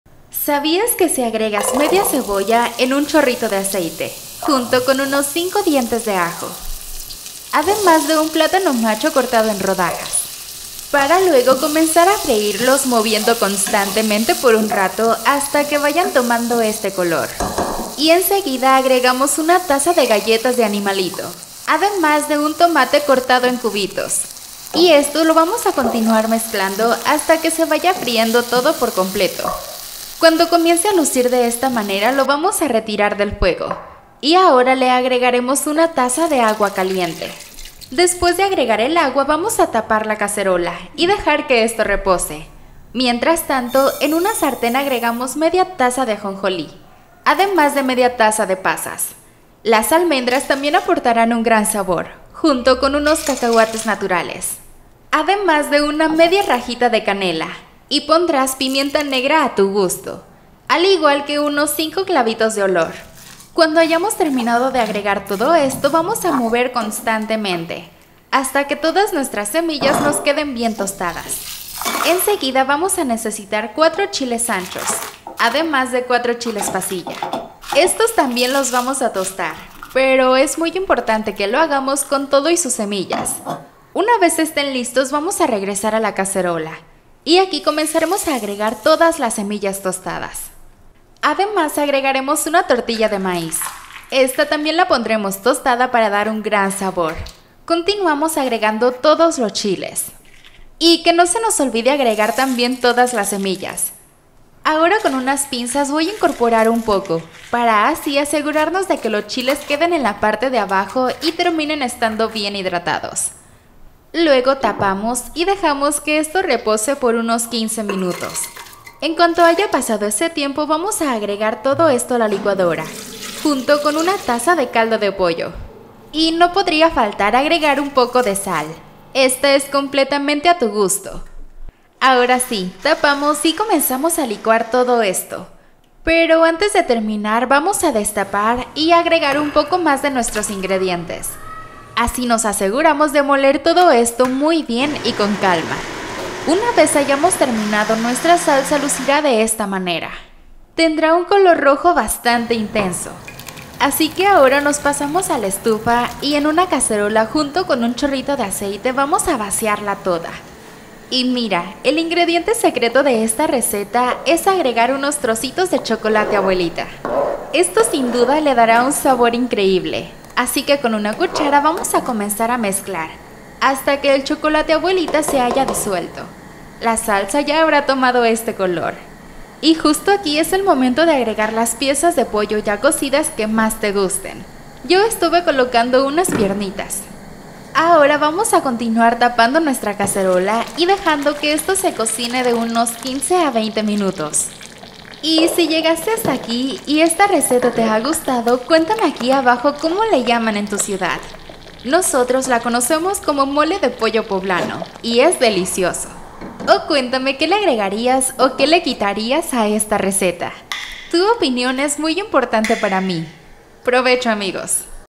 Receta de Mole de Pollo Poblano 😋
En este video te mostrare como hacer un delicioso mole de pollo poblano, de una forma divertida y muy fácil. Cuando lo prepares en casa todos te pedirán la receta 🥰